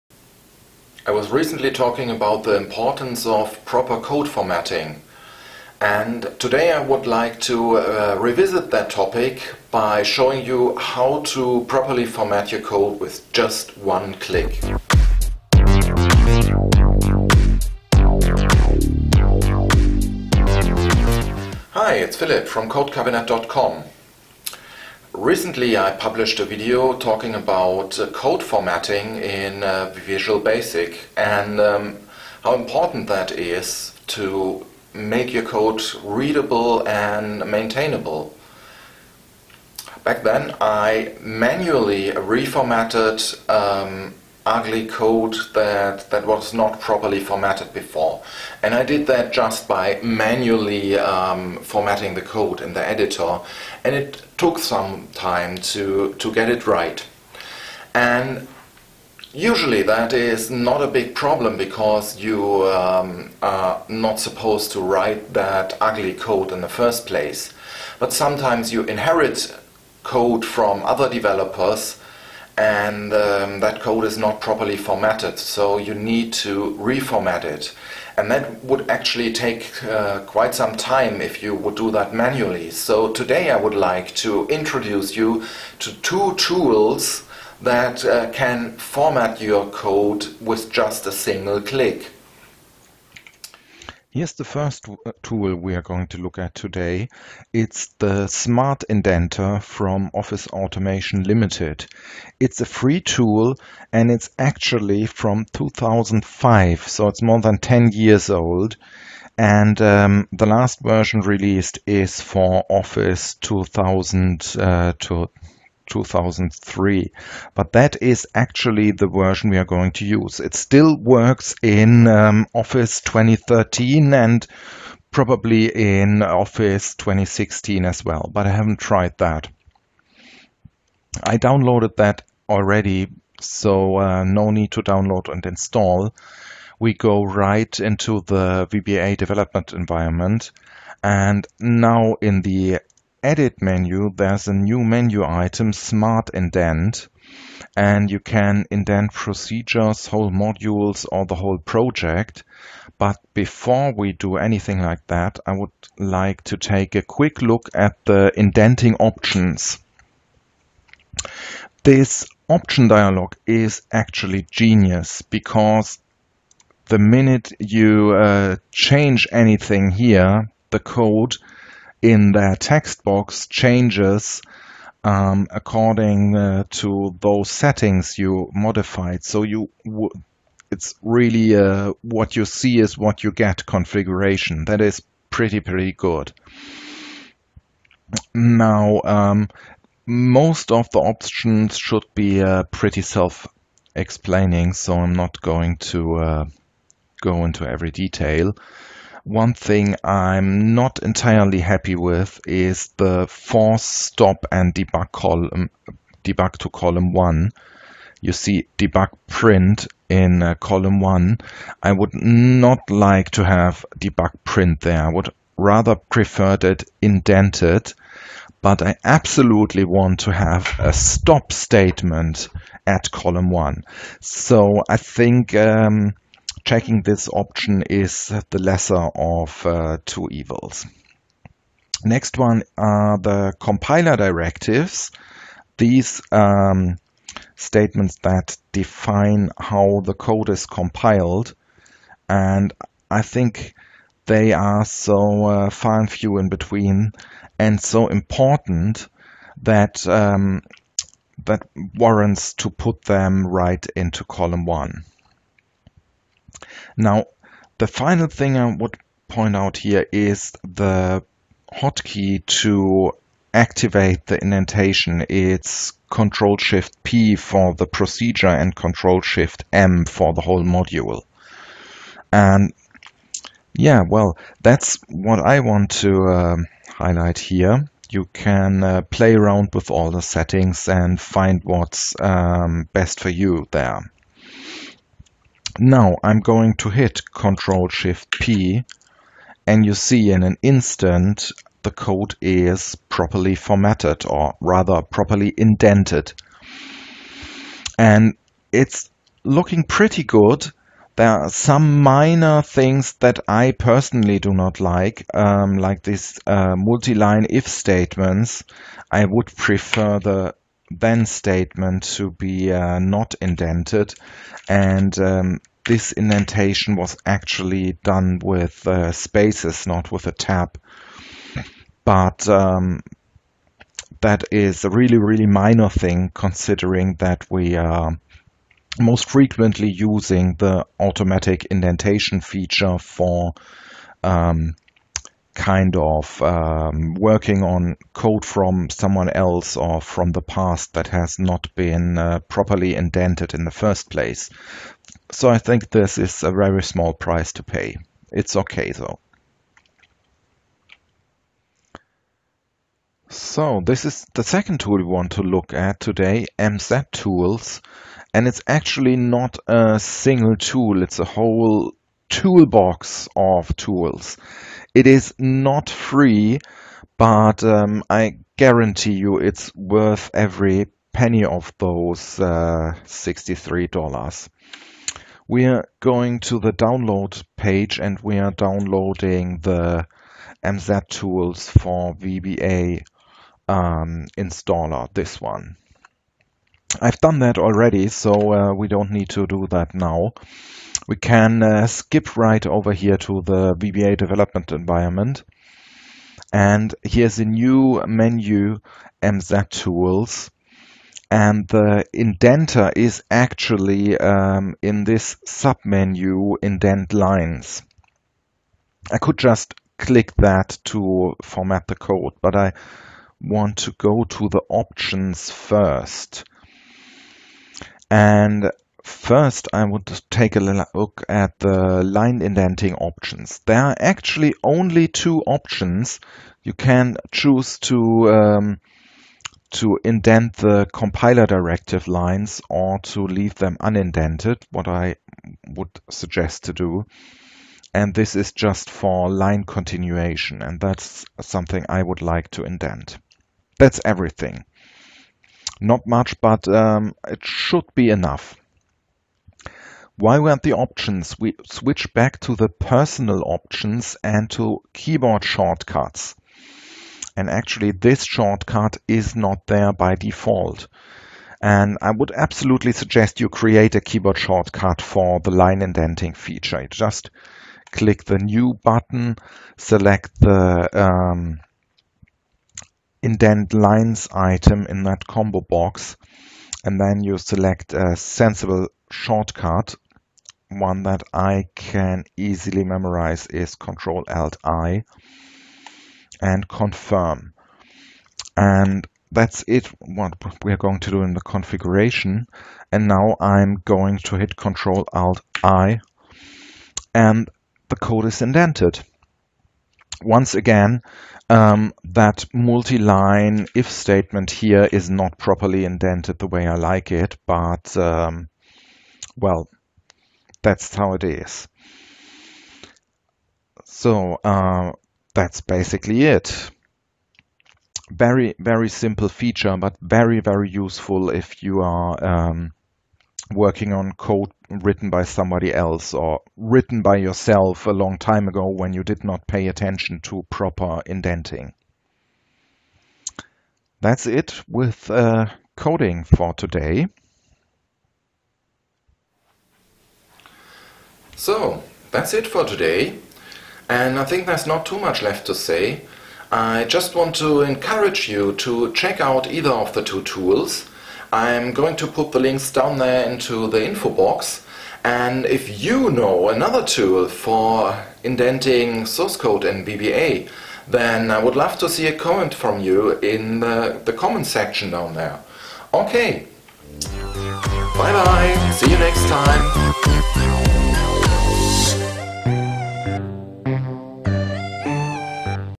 Better VBA 7 - Code formatting tools
Proper formatting of the VBA source code is a crucial factor for the readability of the code. While you are writing code, you should format it correctly right away because it will help you to write solid, error free code.

But what about maintaining legacy applications with badly formatted code? Of course, fixing the formatting of the code has massive benefits through improved readability, but reformatting the code manually takes a lot of effort in a big application.

In this video I show two tools that reduce the effort to reformat VBA source code to virtually zero.

Smart Indenter is a free add-in with the single purpose of indenting VB/VBA source code.

MZ-Tools, an add-in containing a whole suite of useful extensions to Visual Studio and the VBA development environment, has a feature to properly indent source code.

And there is another code indenting tool I discovered only after recording the video. AET VBE Tools - its for Excel VBA only, though.

Here is another video about important elements of good code formatting and the difference between good and poorly formatted code